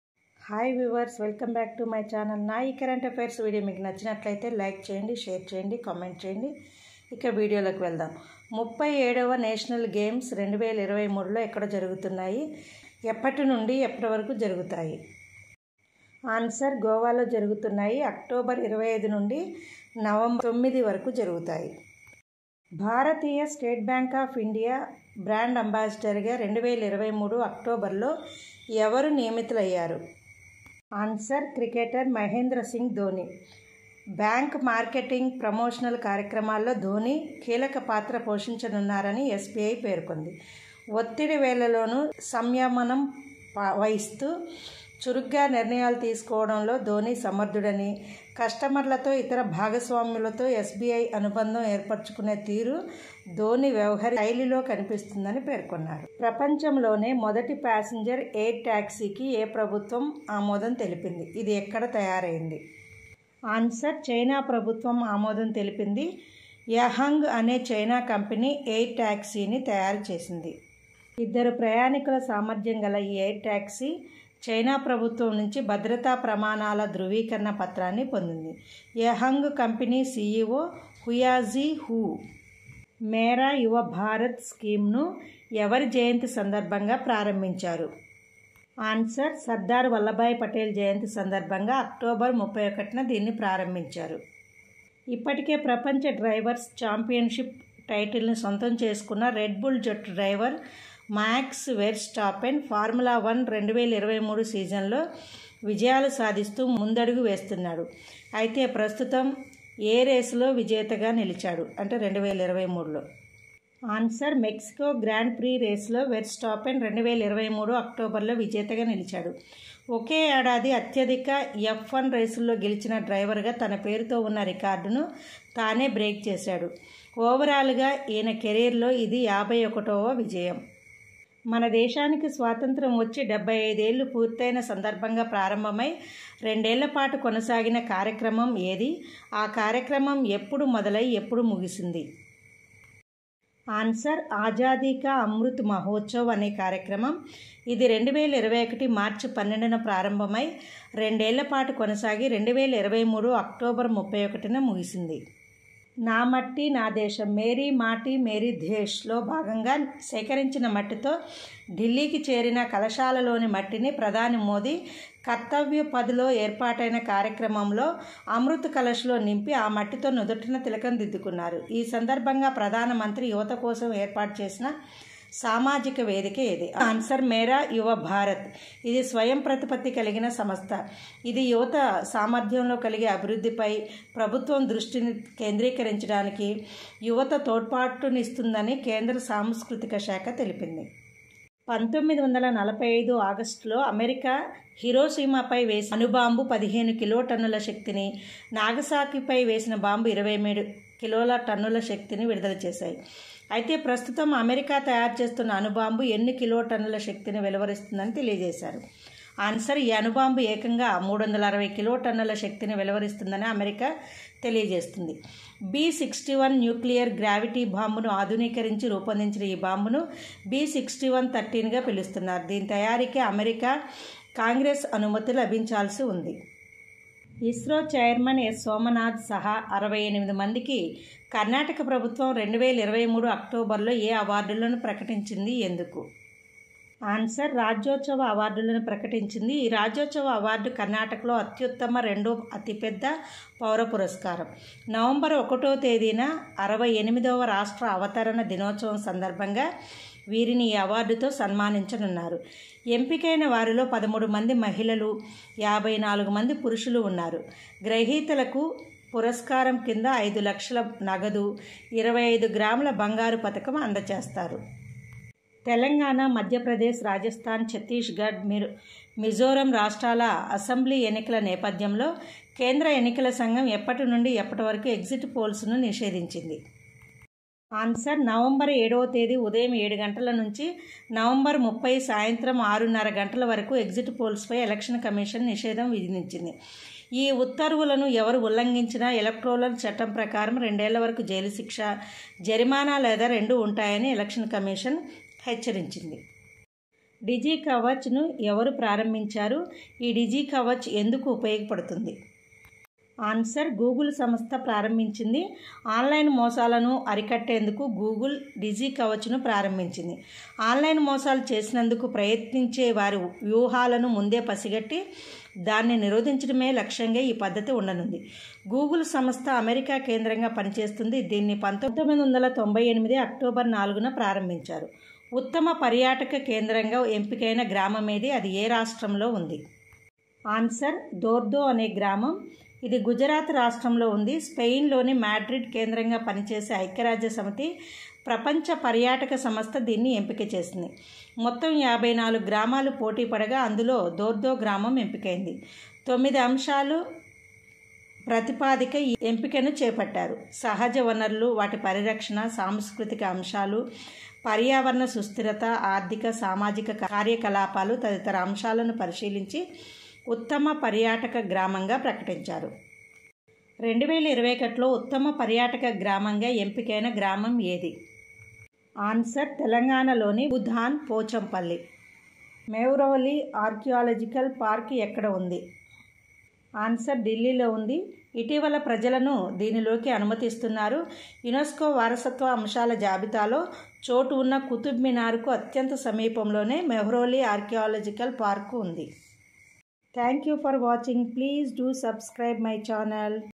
current affairs 2023, October 30th to November 1st.
# 37th National games#
SBI brand ambassador #
passenger air taxy #
Mera Yuva Bharath shceam #
Mexico grand pre race#
Azadi ka Amrit Mahotsav #
America new nueclier bamb#
raajyoothsava award #
uuththama పర్యాటక గ్రామం#
#Mehrouli archiological park #